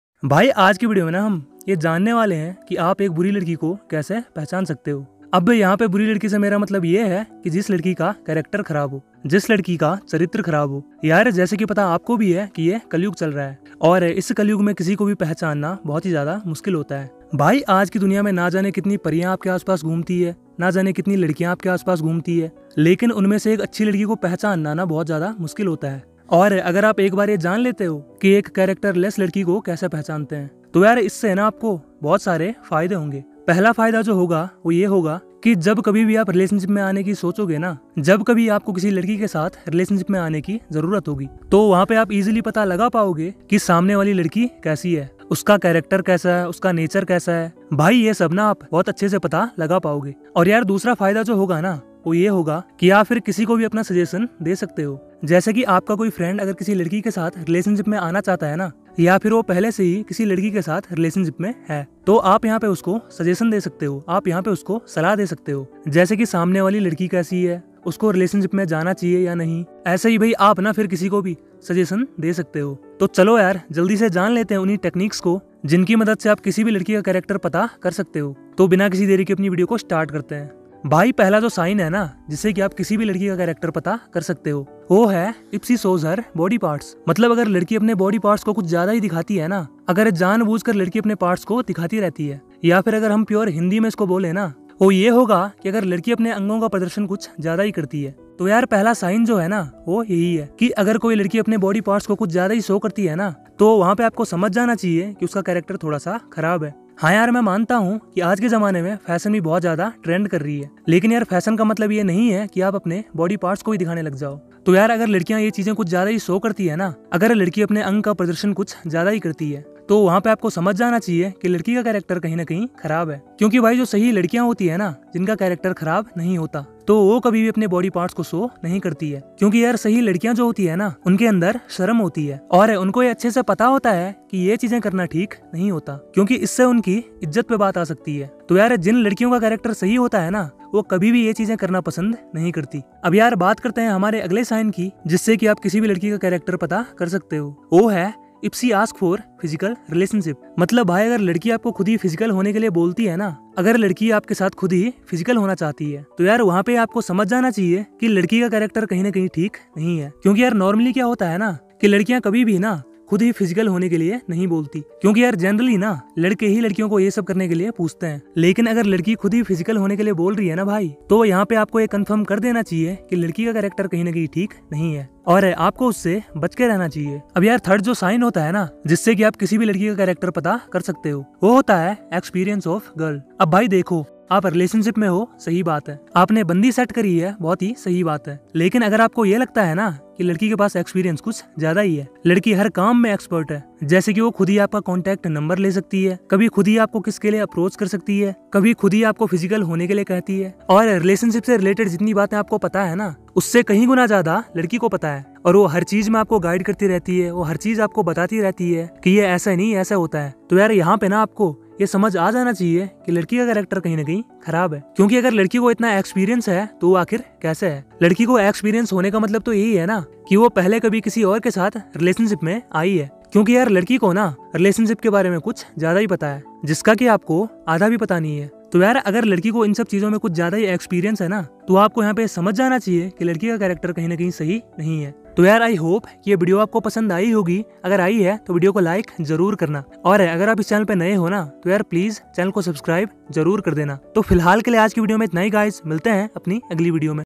अगर ऐसी लड़की कहीं दिखे तो बच के रहना | Buri Ladki ko kaise pahchane | Gandi ladki ki pehchan
About  this Video- ऐसी लड़की कहीं दिखे तो बच के रहना | Buri Ladki ko kaise pahchane | Gandi ladki ki pehchan

is Video me maine aapko bataya hai ki aap Buri ladki ko aap kaise pahchan sakte ho,agr aapko kisi ladki ke sath Relationship me jana hai to aapko ye pata hona chahiye ki ladki theek hai ya nahi.